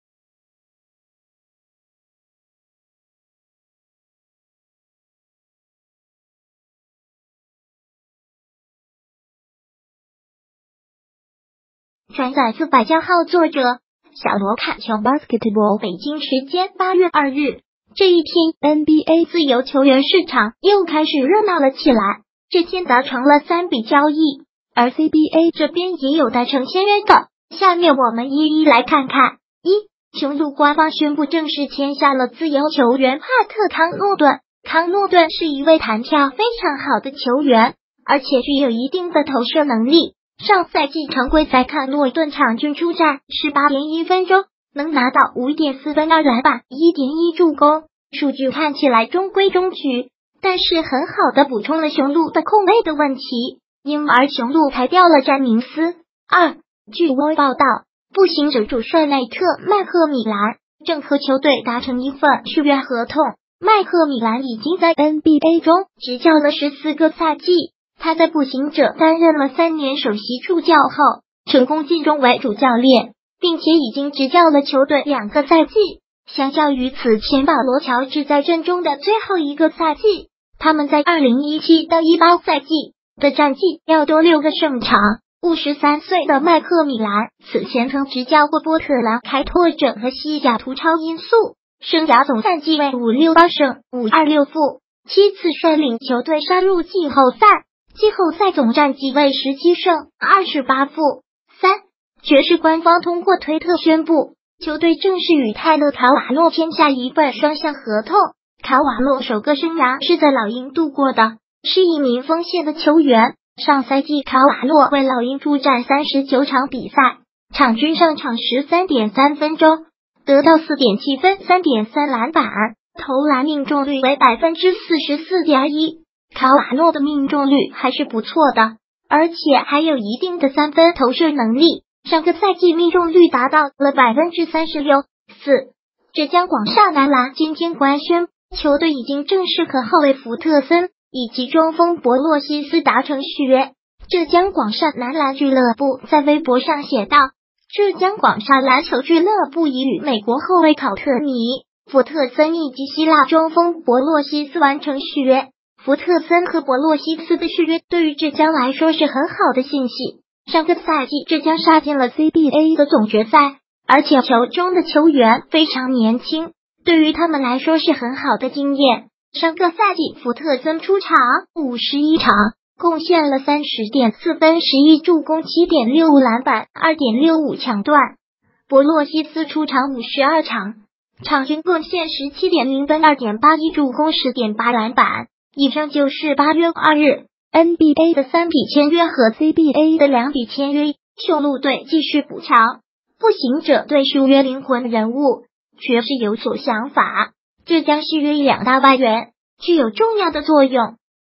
8月第二天，NBA达成3笔签约，CBA达成2笔签约
转载自百家号作者：小罗侃球BASKETBALL北京时间8月2日，这一天NBA自由球员市场又开始热闹了起来，这天达成了3笔交易，而CBA这边也有达成签约的，下面我们一一来看看。1.雄鹿官方宣布正式签下了自由球员帕特-康诺顿。康诺顿是一位弹跳非常好的球员，而且具有一定的投射能力。上赛季常规赛康诺顿场均出战18.1分钟能拿到5.4分2篮板1.1助攻。数据看起来中规中矩，但是很好的补充了雄鹿的控卫的问题，因而雄鹿裁掉了詹宁斯。2.据Woj报道，步行者主帅内特-麦克米兰正和球队达成一份续约合同。麦克米兰已经在NBA中执教了14个赛季，他在步行者担任了三年首席助教后，成功晋升为主教练，并且已经执教了球队两个赛季。相较于此前保罗-乔治在阵中的最后一个赛季，他们在2017-18赛季的战绩要多六个胜场。53岁的麦克米兰此前曾执教过波特兰开拓者和西雅图超音速，生涯总战绩为568胜526负，七次率领球队杀入季后赛，季后赛总战绩为17胜28负。3.爵士官方通过推特宣布，球队正式与泰勒-卡瓦诺签下一份双向合同。卡瓦诺首个生涯是在老鹰度过的，是一名锋线的球员，上赛季卡瓦诺为老鹰出战39场比赛，场均上场13.3分钟